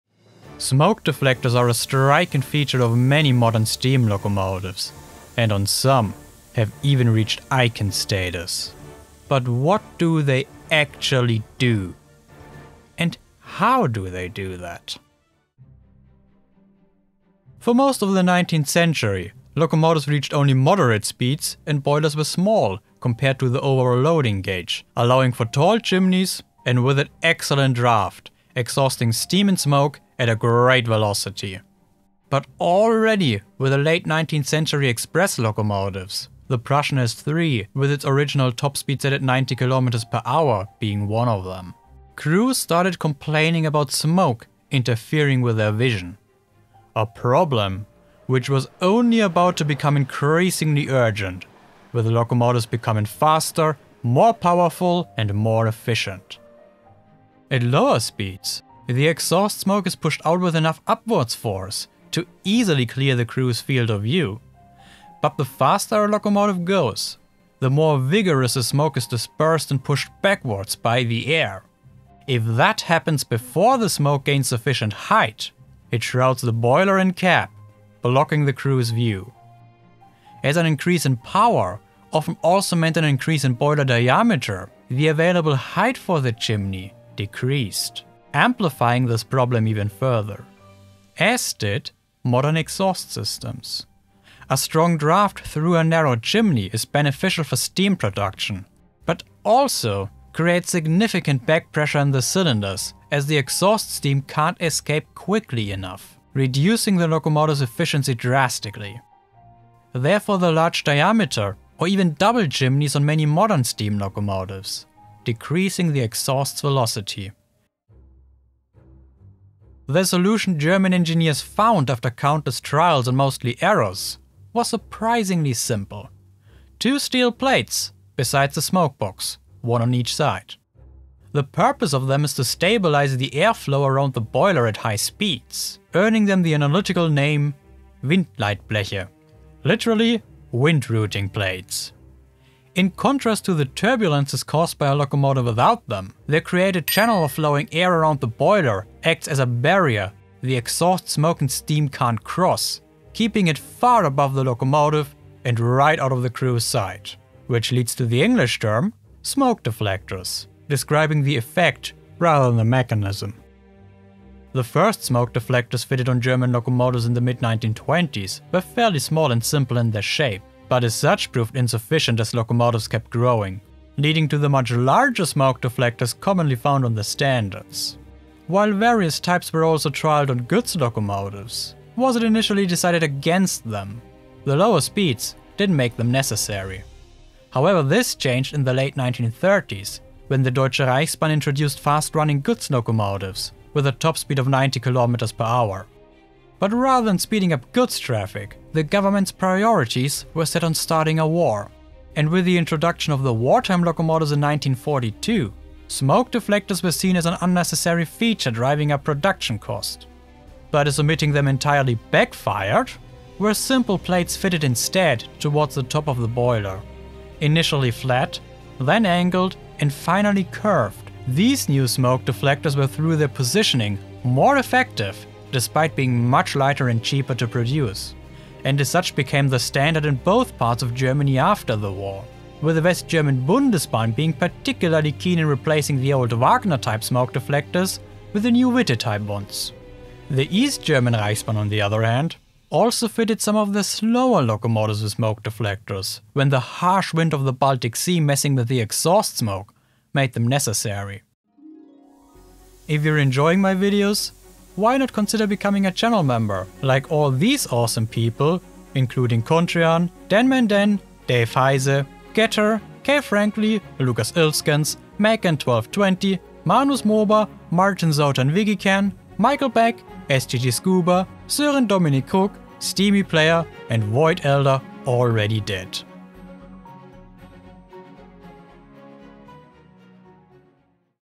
What do they actually do?
Since the 1920s, smoke deflectors have become a vital part of many steam locomotives in Germany and all around the world. But what is their purpose, and how do they work?

All models in this video were created, rendered, and animated by myself using Blender.